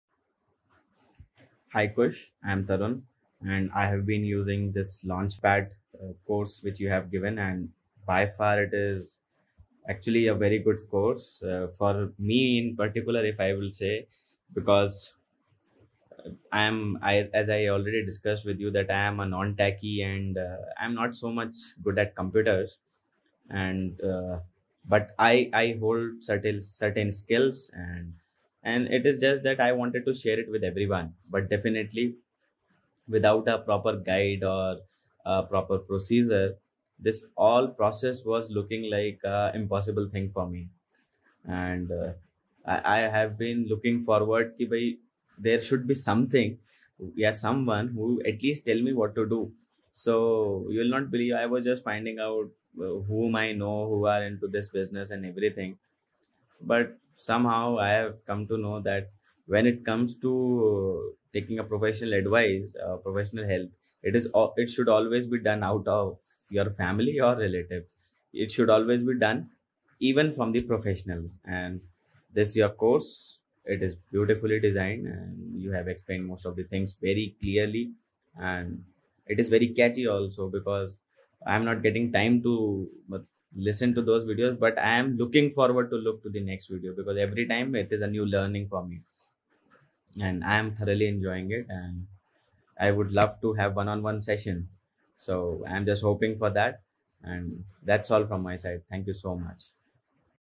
Student Review - Tarun Pratap Singh
Here's what our student Tarun Pratap Singh had to say about our course called Online Course Launchpad, which teaches you the A to Z of creating and selling your first ever Online Course and starting a successful Online Coaching Business.